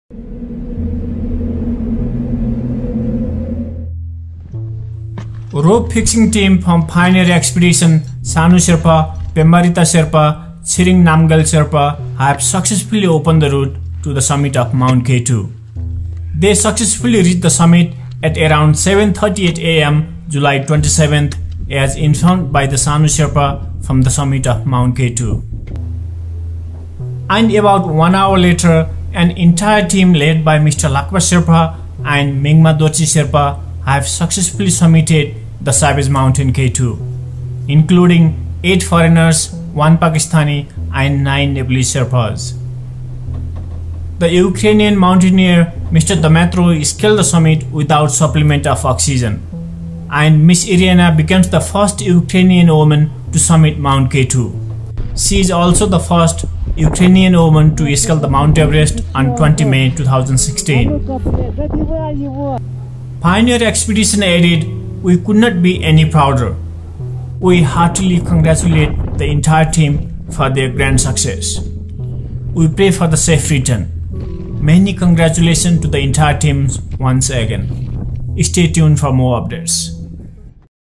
K2 2021 - THE SHERPAS & TEAM SCALED MT K2 | IRINA BACAME FIRST UKARINAN WOMAN TO SCLAED K2
MT k2 summer expedition successfully scaled by Nepalese sherpas and their team members. Pioneer expedition rope fixing team Mr. Sanu Sherpa, Mr. Pemba Rita Sherpa, Mr. Chhiring Namgel Sherpa have successfully opened the route to the summit of mt k2!

They successfully reached the summit at around 7:38 am July 27th as informed by Mr. Sanu Sherpa from the summit of Mt K2.

And about 1 hour later an entire team led by Mr. Lakpa Sherpa and Mr. Mingma Dorchi Sherpa have successfully summited the Savage Mountain K2. Including 8 foreigners, 1 Pakistani, and 9 Nepalese sherpas. Mr. Dmytro scaled the summit without supplement of o2, and MS Iryna becomes the first Ukrainian woman to summit K2, she is also the first Ukrainian woman to scale Mt Everest on 20 May 2016

Pioneer expedition added we couldn't be any prouder. We heartily congratulate the entire team for their grand success.
We pray for their safe return
Many congratulations to the entire team once again!

Thanks and Namaste